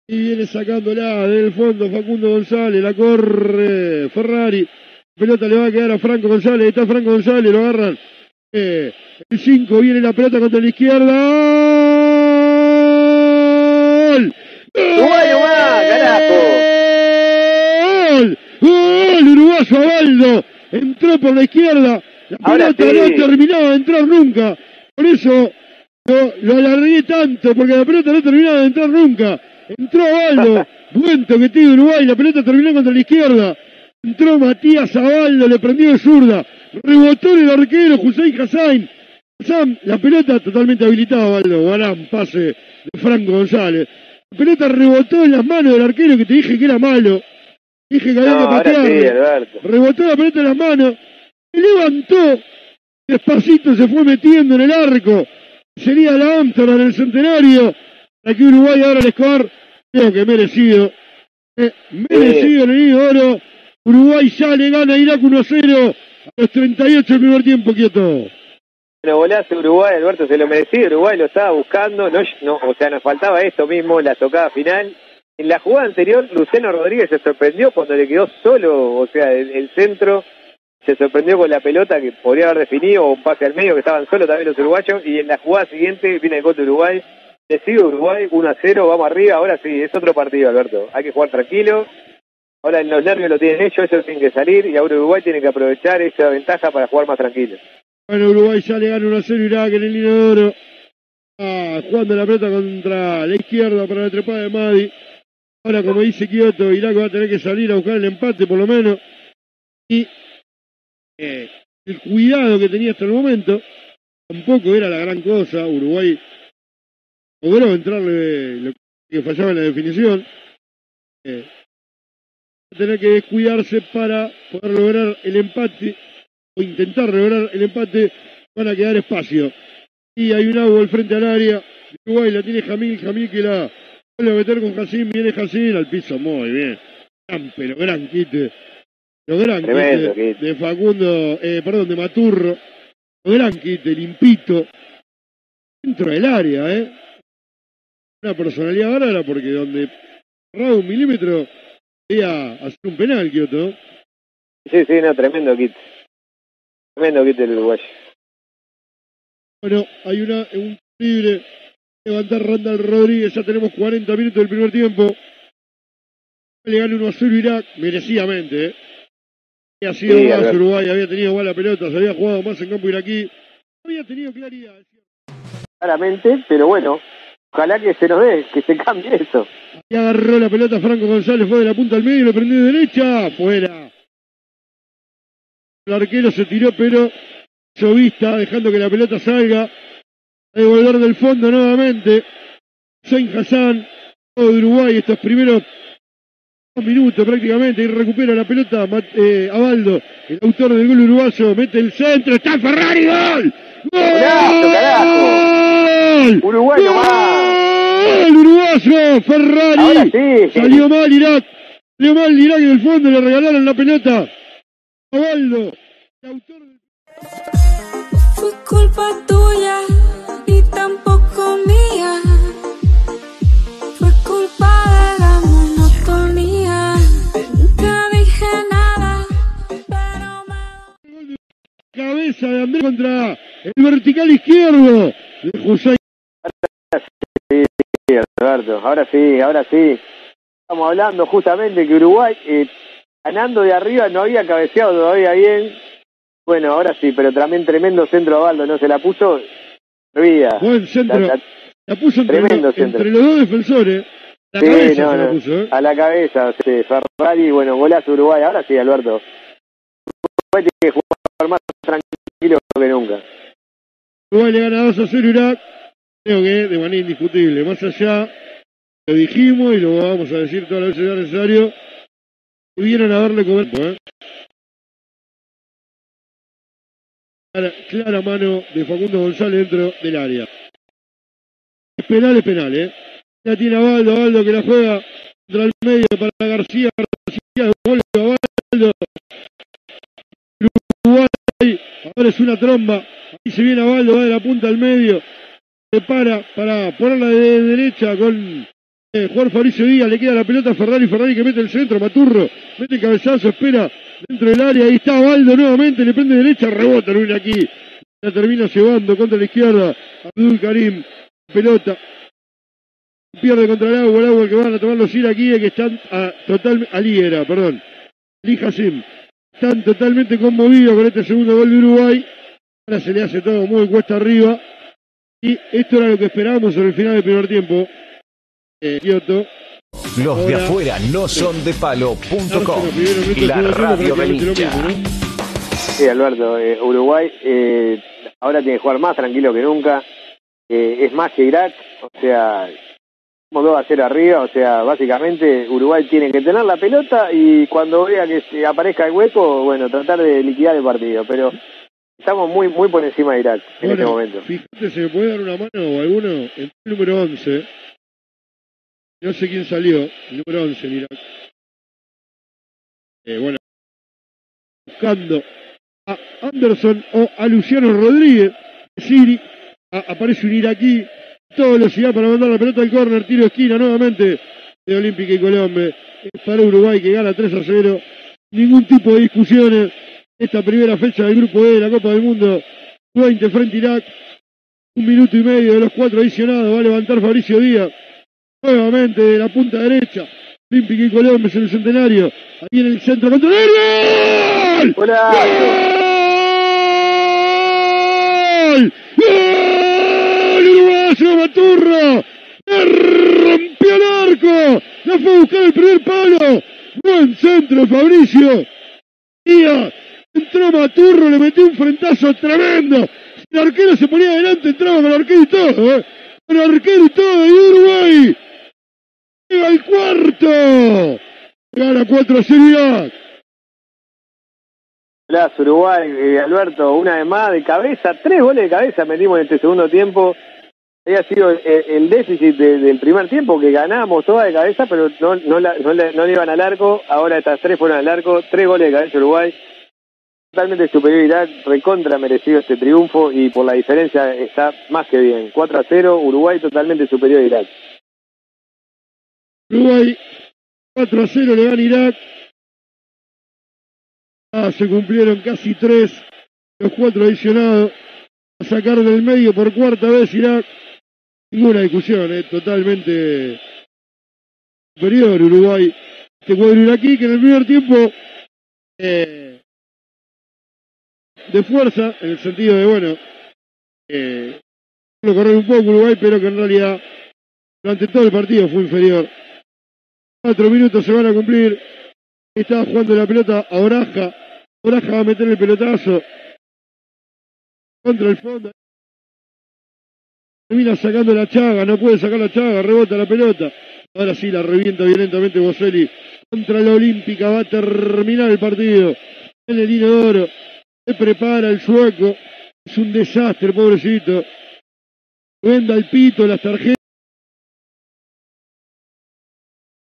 Uruguay 4 Irak 0 - Copa del Mundo Sub-20 2023 (por Alberto Raimundi)
Los goles del partido (falta el tercero por problemas técnicos), por Alberto Raimundi para Revolución FM de Argentina.

Grabación de la retransmisión de @losdeafueranosondepalo